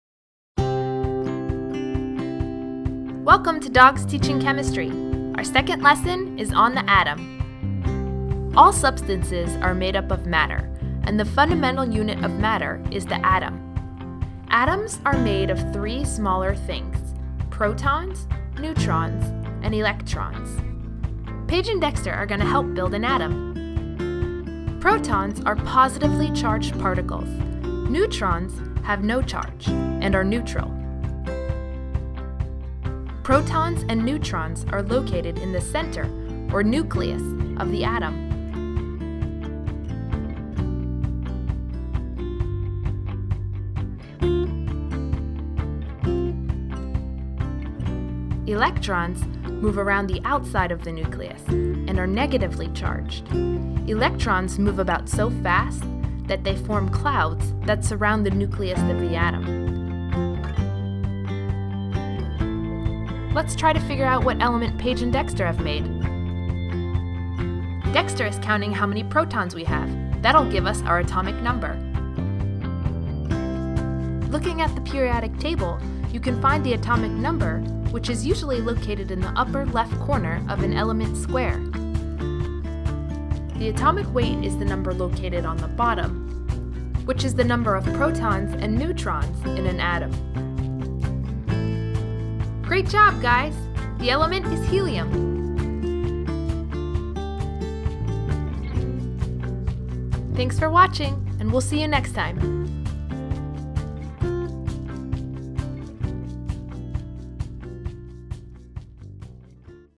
Dogs Teaching Chemistry - The Atom
I am a chemist and my greatest hobby is to train my dogs...why not combine the two?! Dexter's facebook Paige's facebook

All substances are made up of matter and the fundamental unit of matter is the atom.

Elements are represented as the squares you see on the periodic table. Each elements consists of one type of atom

Atoms are made of of 3 smaller things:
• Protons
• Neutrons
• Electrons
Protons and Neutrons are located in the center, or nucleus, of an atom
Protons are positively charged (+),
Neutrons  have no charge and are neutral
Electrons move around outside the nucleus and are negatively charged (-)
Electrons move about so fast, that they form clouds that surround the nucleus of an atom. Those clouds are specific distances away from the nucleus and are generally organized into shells.

The atomic number is the number of protons an atom has. the number of protons an atom has determines its identity. So the atomic number is unique for each element.

The atomic weight (also referred to as the atomic mass) is the number of protons and neutrons in an atom.
On the periodic table, The atomic number is the number usually located in the upper left corner of an element's square. The atomic weight is the number located on the bottom.